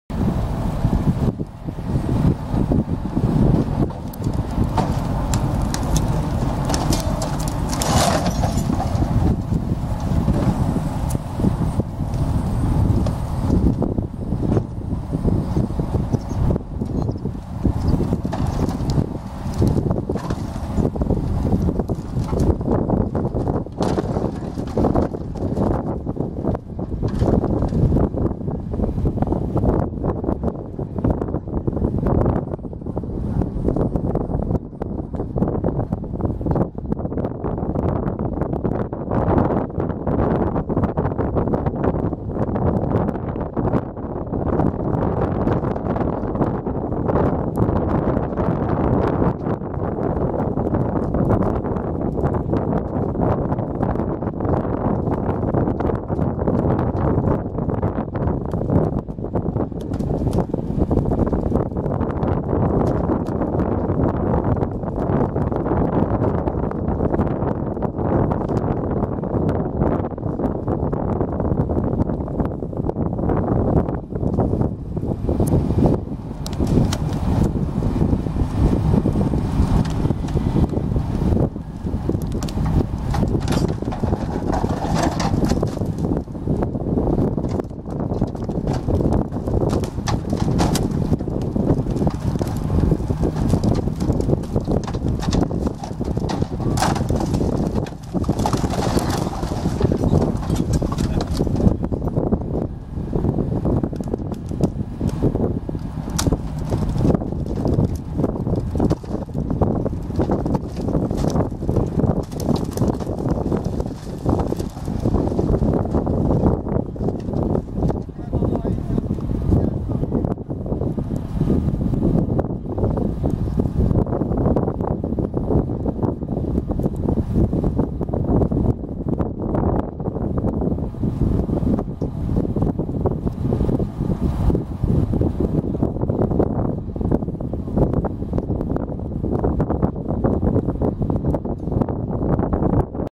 В Астрахани уплывают дома.
В Астрахани половодье,подмывает берега и дома падают в реку.